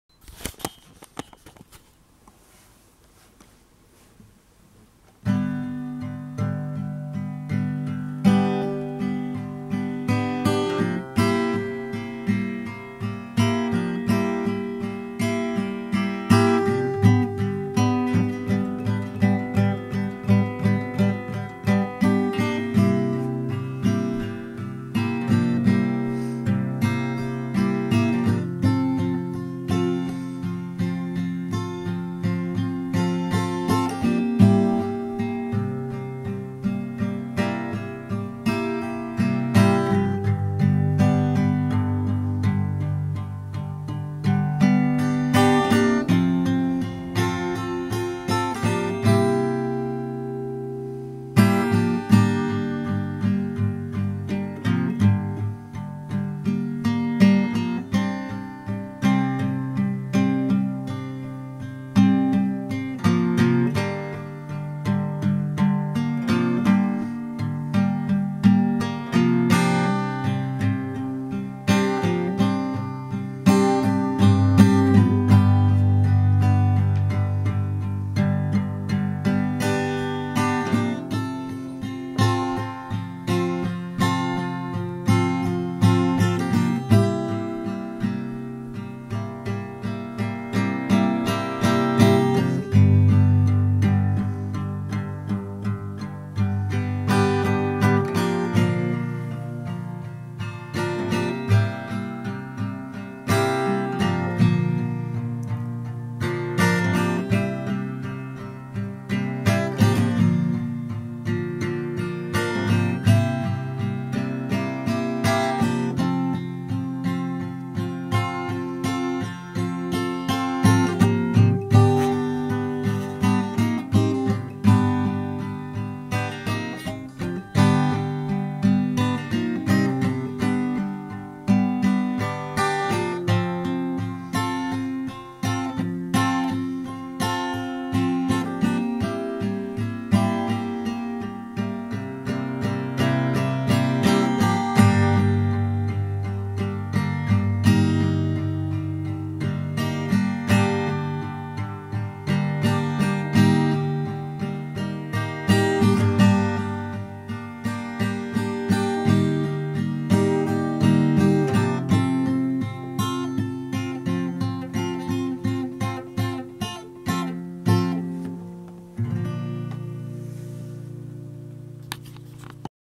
strumming for strummings sake by graham byrne
just having a wee strum,strum,strum...strum along if your bored.